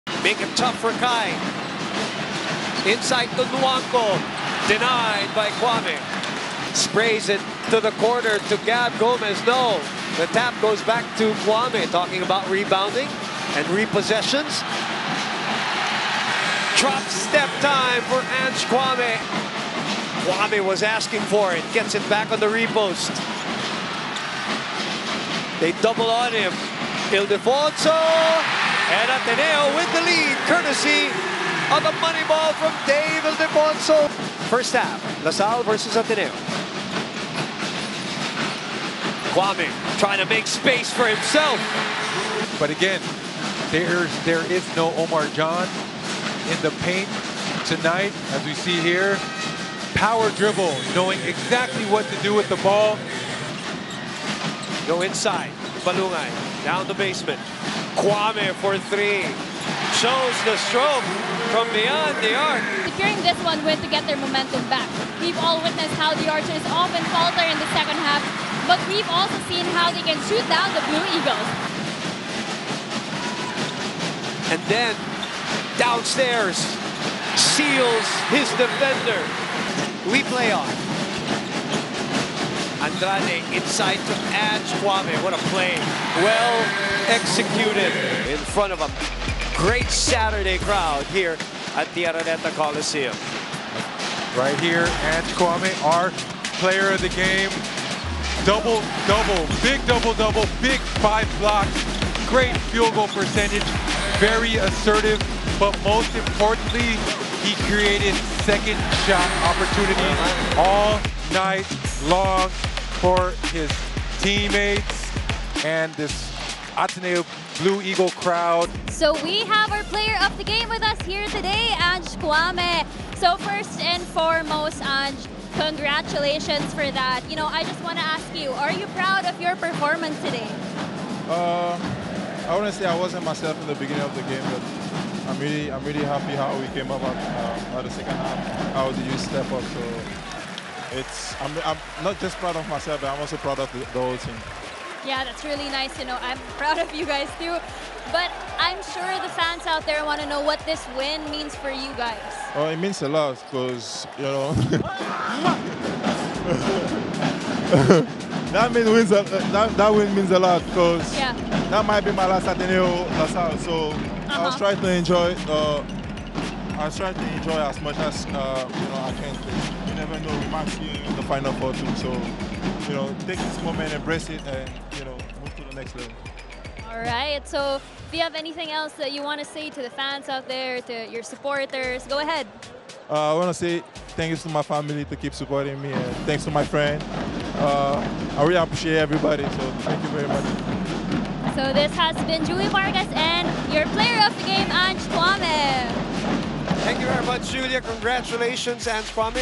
Ange Kouame unleashed against DLSU | UAAP Season 85 Men's Basketball
Ange Kouame in full MVP form as Ateneo takes its win back from DLSU!